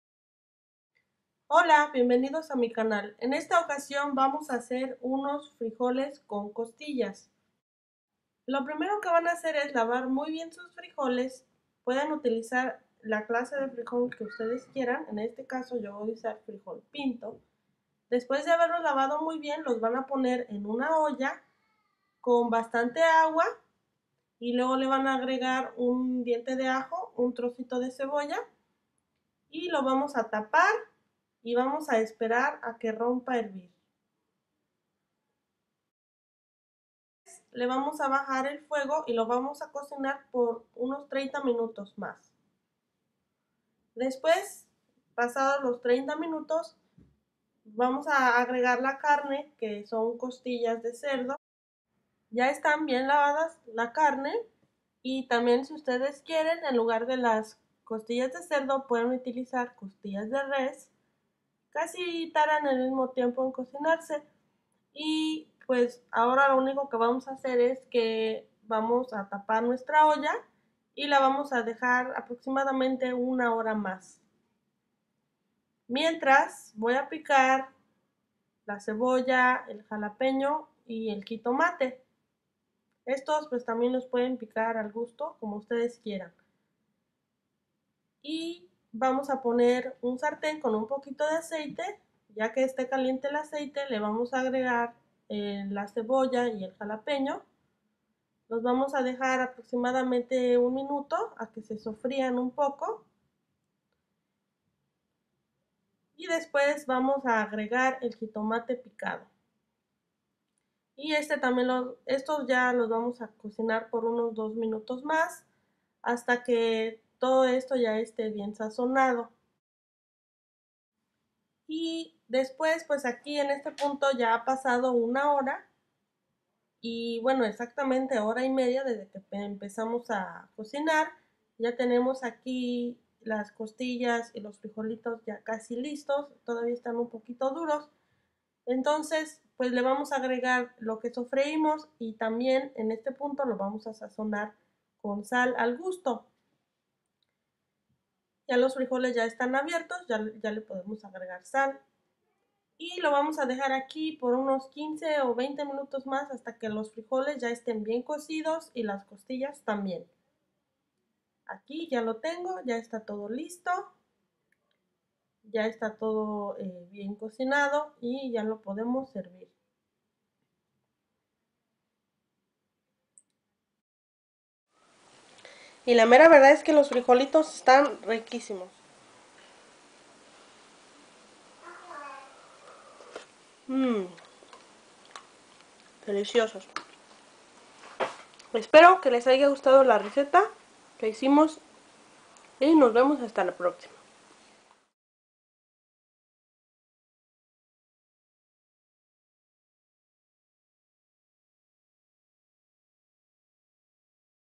Cómo cocinar unos ricos frijoles con costillas - La receta de la abuelita
A petición de fafaelito1 les traigo esta receta de unos ricos frijoles con costillas. Espero que sea la correcta, ya que le tuve que preguntar a mi tía Mari que sabe de cocina de oaxaca y así me dio la receta, espero que les guste.

INGREDIENTES:
1lb o 450 grs de frijol de su preferencia
1 jitomate
1 cebolla blanca
1 diente de ajo
jalapeños al gusto

Bistec en salsa de pasilla